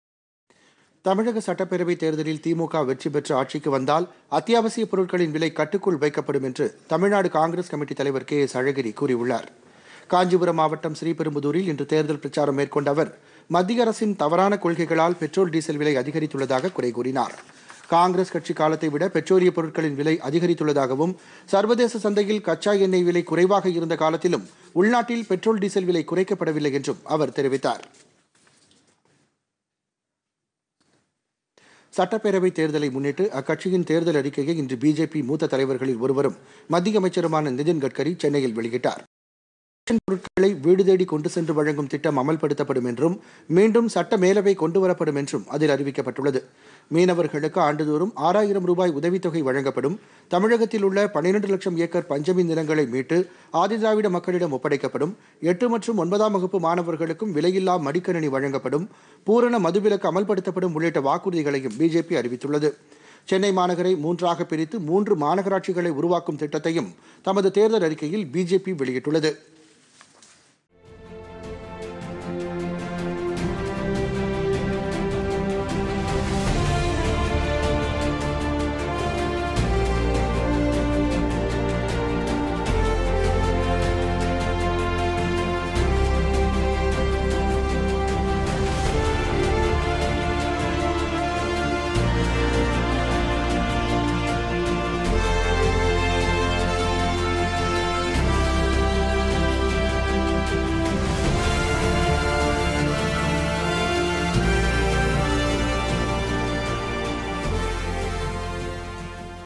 பிஜேபி தேர்தல் அறிக்கை வெளியீடு
தமிழகத்தில் ரேஷன் பொருட்கள் வீடு தேடி வழங்கப்படும்  - சட்ட மேலவை மீண்டும் கொண்டு வரப்படும் - பிஜேபி தேர்தல் அறிக்கையை வெளியிட்டார் மத்திய அமைச்சர் நிதின் கட்கரி